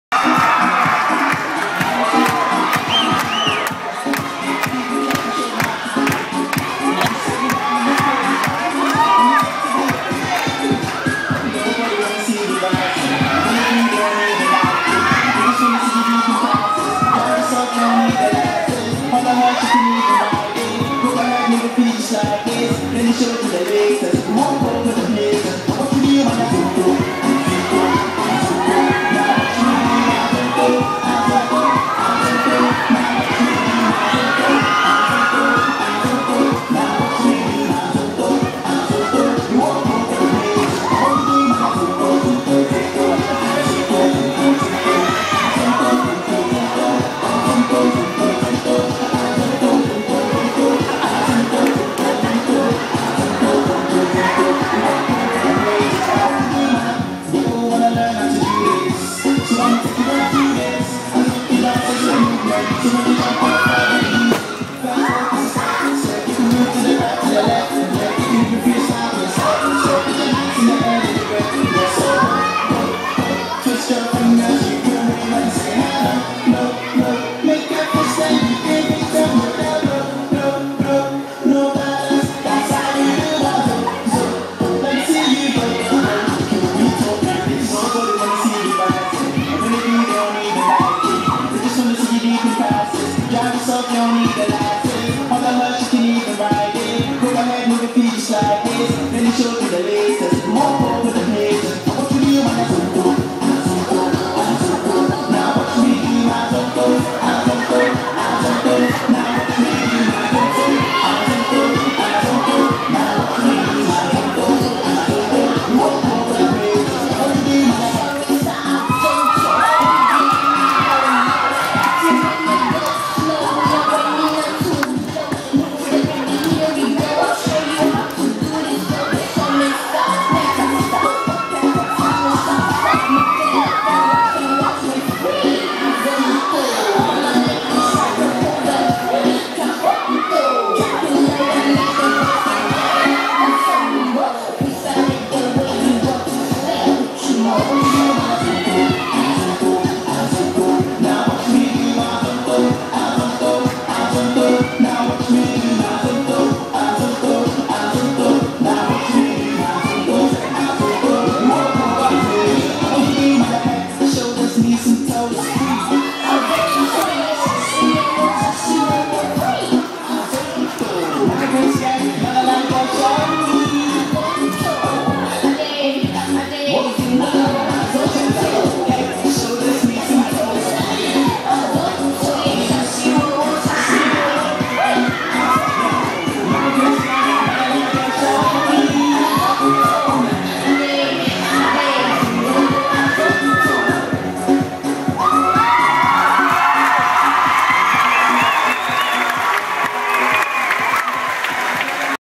Chevalier College Mission Concert 2012
Kiribati 2012 trip participants doing an i-Kiribati dance